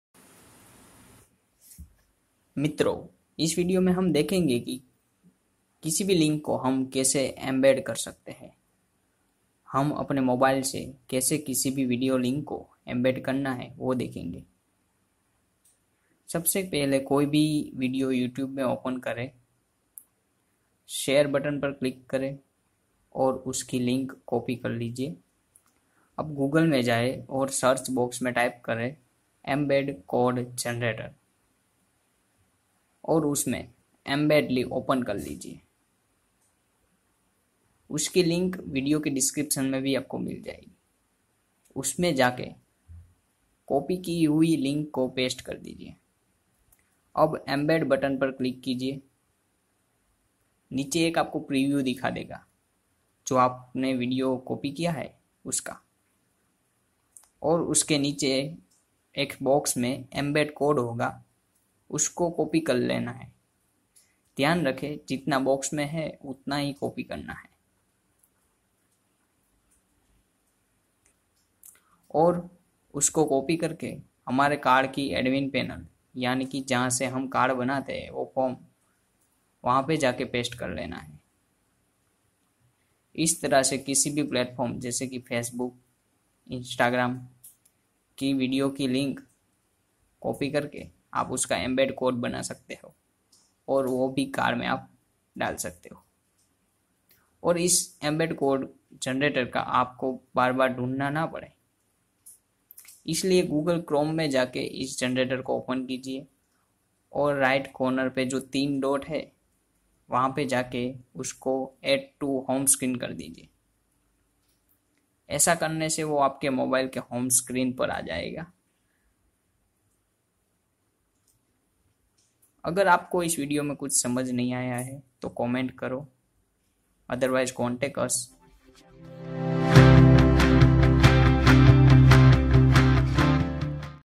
How To Generate Embed Code From Any Link? | Link To HTML Embed Code Generator In Mobile
How To Generate Embed Code From Any Link? | Link To HTML Embed Code Generator From Mobile Device

This Video is About Generation Of Embed Code From video link.
Which is useful in any website or web design to show video.